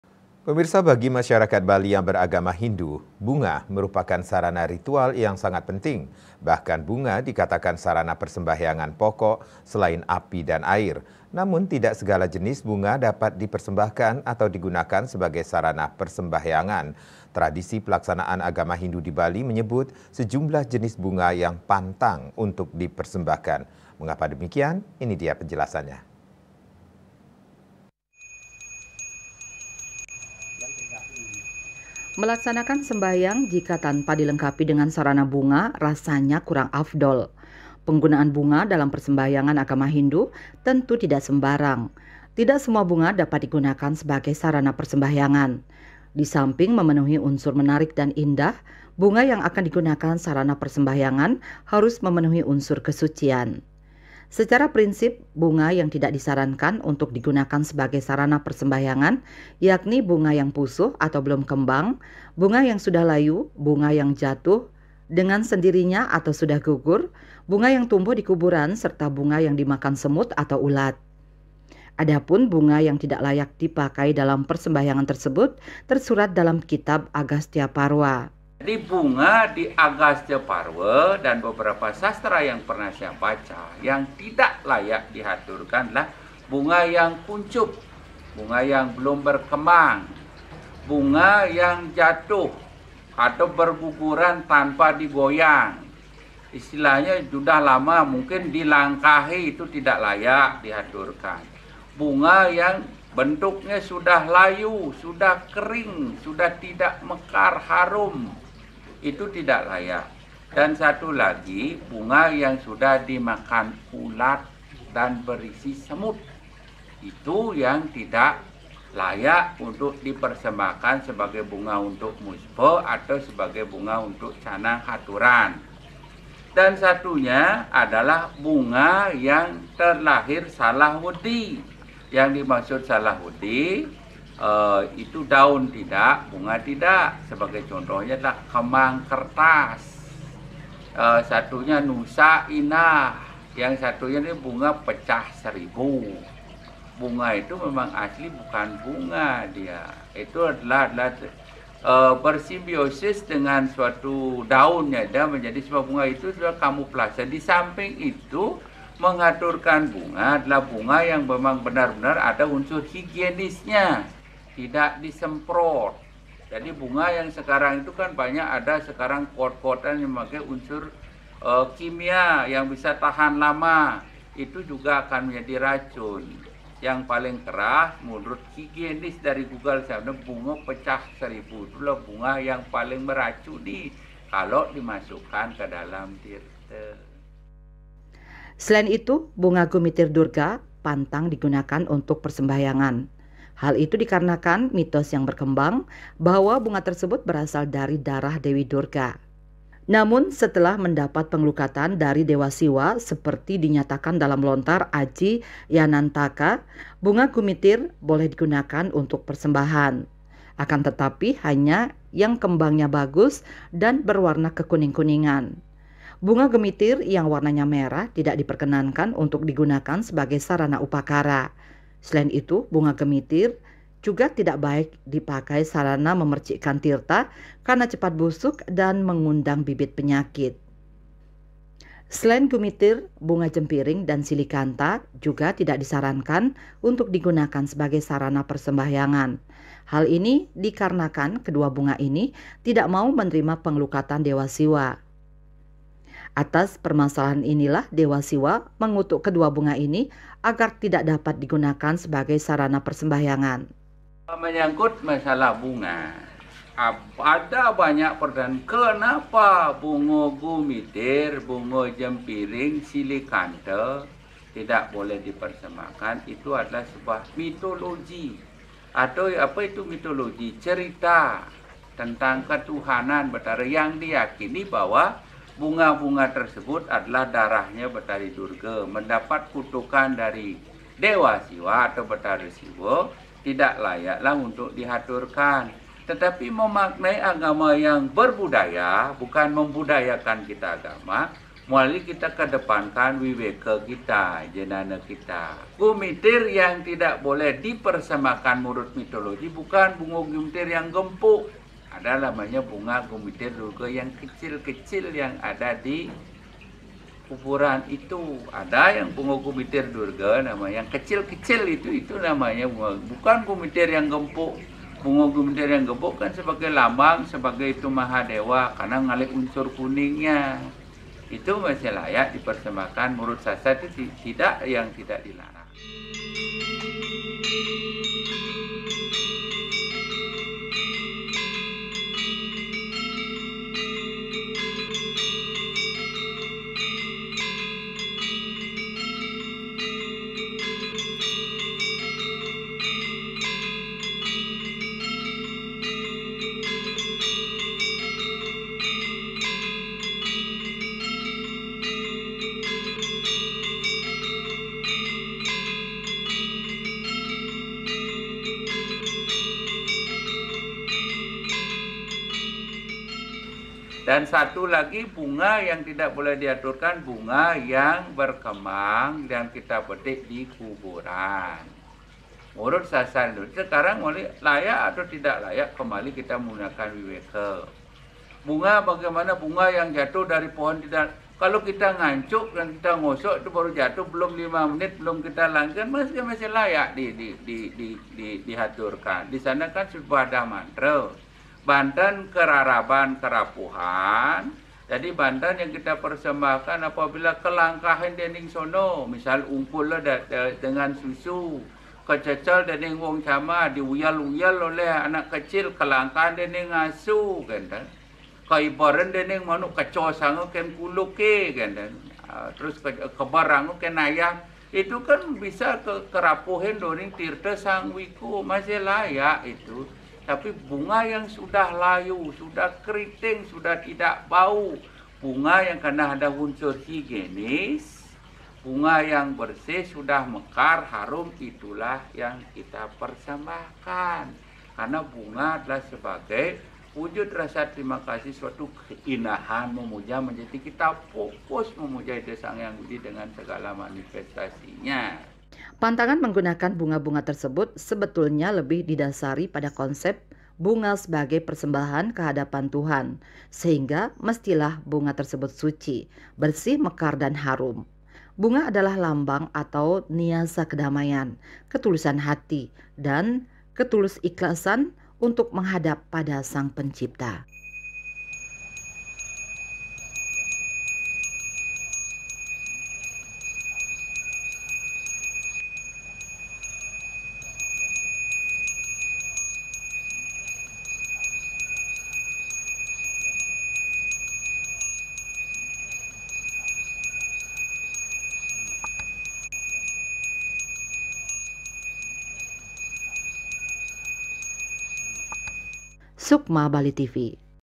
BUNGA TERLARANG UNTUK SEMBAHYANG MENURUT HINDU, INI PENJELASANNYA
BAGI MASYARAKAT  BALI YANG BERAGAMA HINDU, BUNGA MERUPAKAN SARANA RITUAL YANG SANGAT PENTING.  BAHKAN BUNGA DIKATAKAN SARANA PERSEMBAHYANGAN POKOK, SELAIN API DAN AIR. NAMUN, TIDAK SEGALA JENIS BUNGA DAPAT DIPERSEMBAHKAN, ATAU DIGUNAKAN SEBAGAI SARANA PERSEMBAHYANGAN. TRADISI PELAKSANAAN AGAMA HINDU DI BALI MENYEBUT SEJUMLAH JENIS BUNGA YANG PANTANG UNTUK DIPERSEMBAHKAN. MENGAPA DEMIKIAN?. INI DIA PENJELASANNYA.